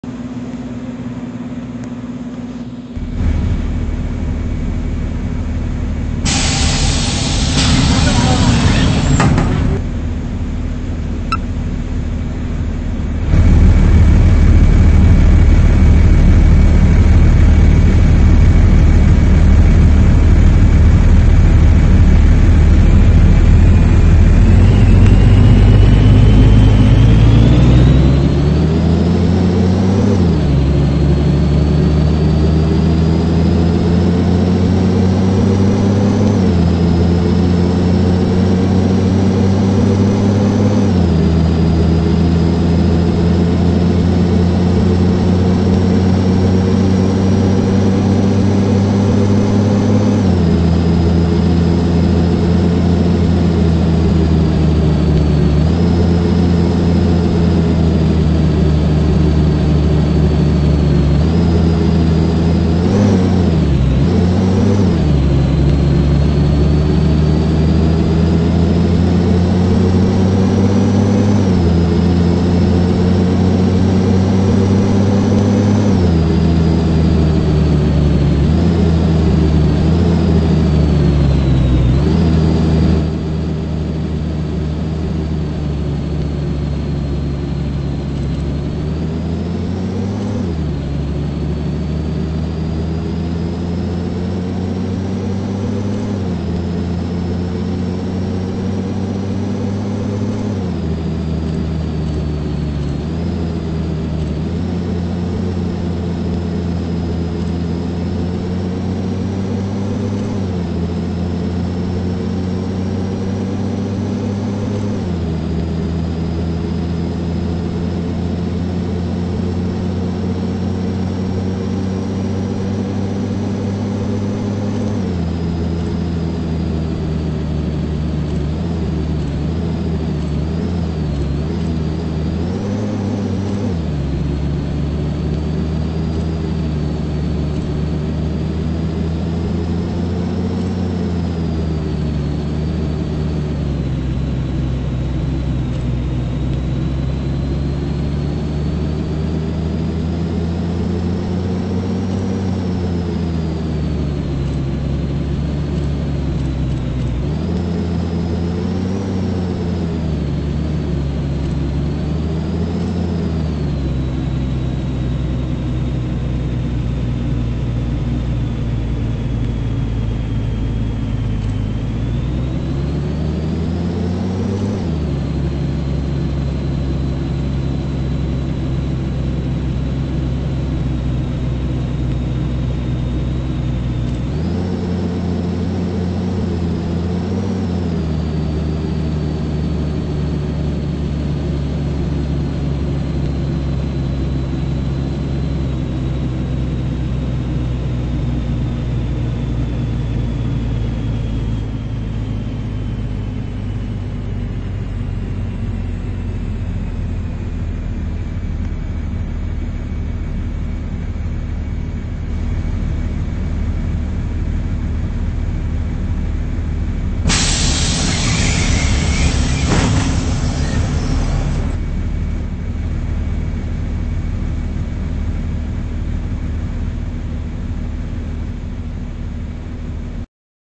Microbaires Argentina (Leer Descripción)
Disculpen si manejo mal recién hace poco empecé a manejar Bondis
Video Grabado el Día 15/04/2021 a las 08 AM